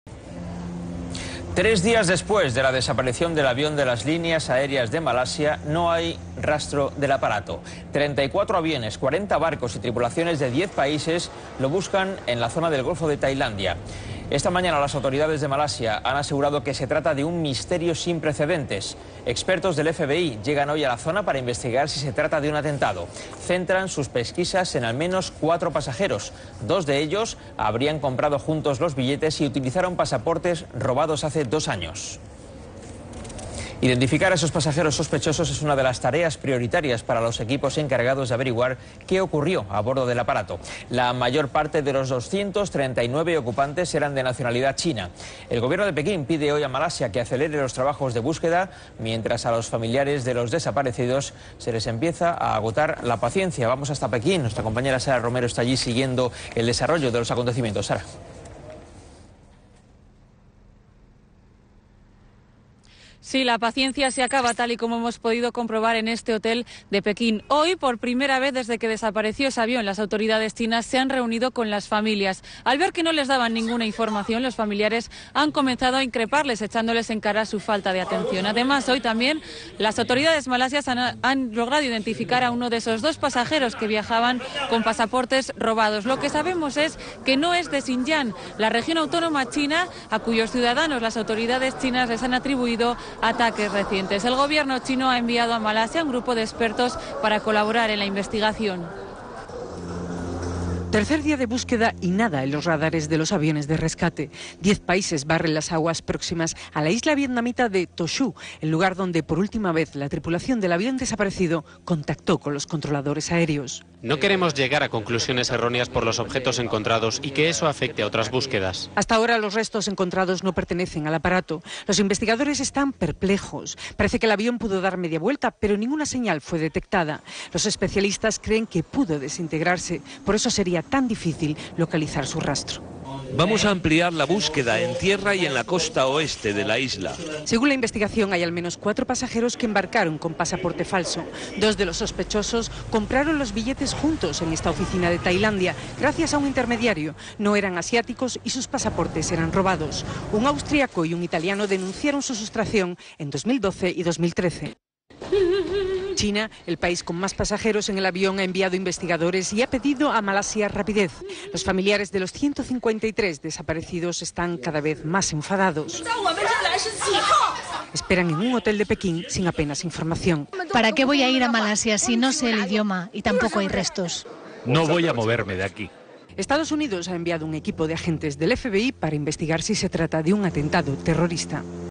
Las autoridades vietnamitas descartan que el objeto amarillo sea una lancha del avión desaparecido
Un helicóptero vietnamita que participa en las labores de rescate del avión Boeing 777 de Malaysian Airlines desparecido el sábado ha descartado este lunes que el objeto amarillo que había sido avistado en aguas vietnamitas sea una lancha de emergencia de la aeronave, ha informado la Autoridad de Aviación Civil de Vietnam.